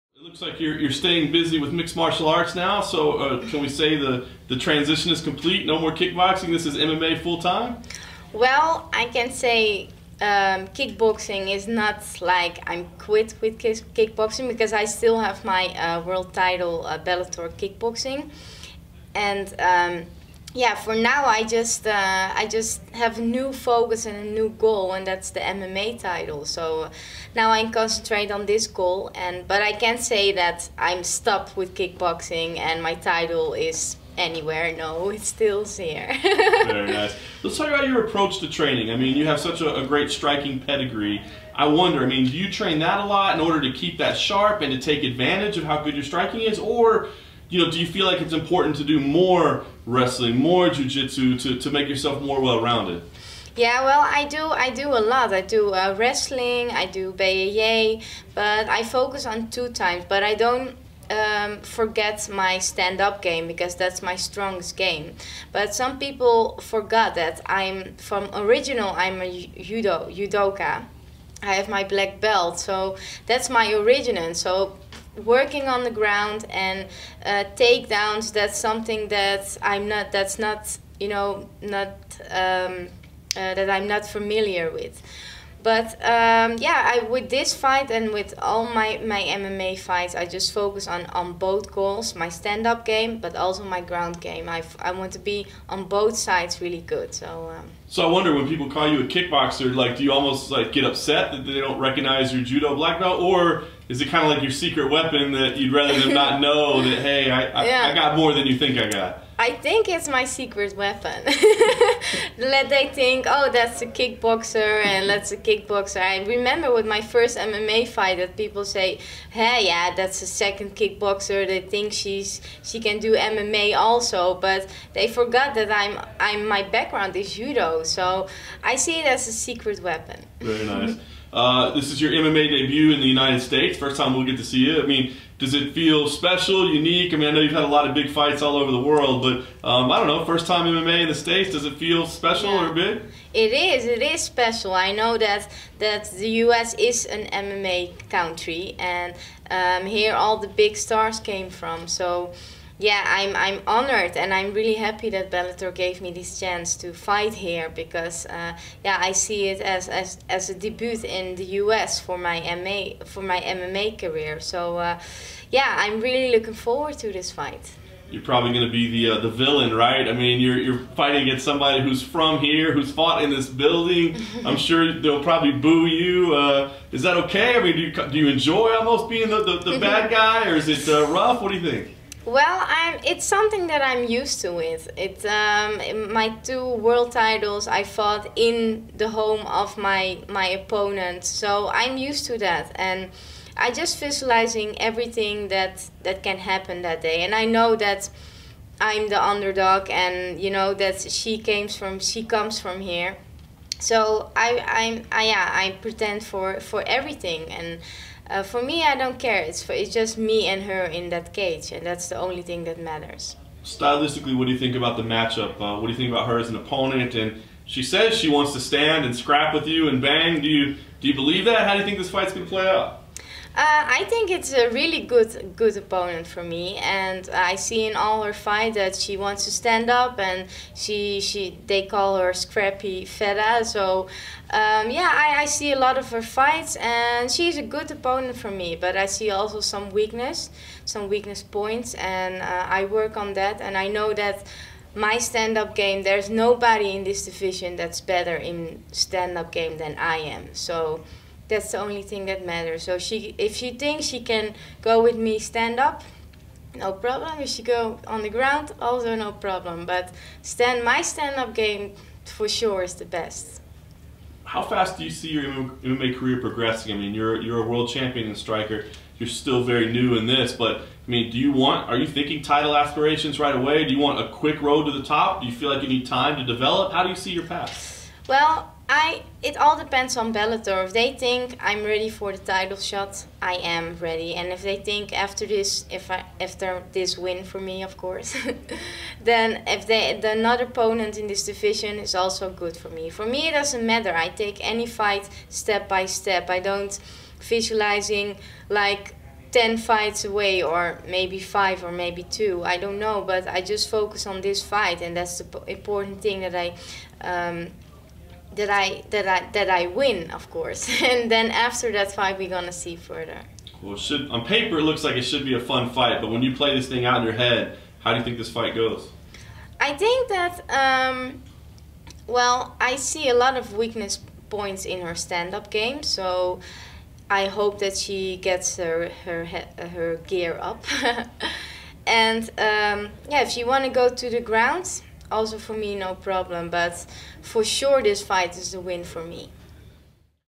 Bellator 205: Denise Kielholtz full pre-fight interview
Bellator women's flyweight Denise Kielholtz spoke to MMAjunkie ahead of Friday's Bellator 205 event in Boise, Idaho.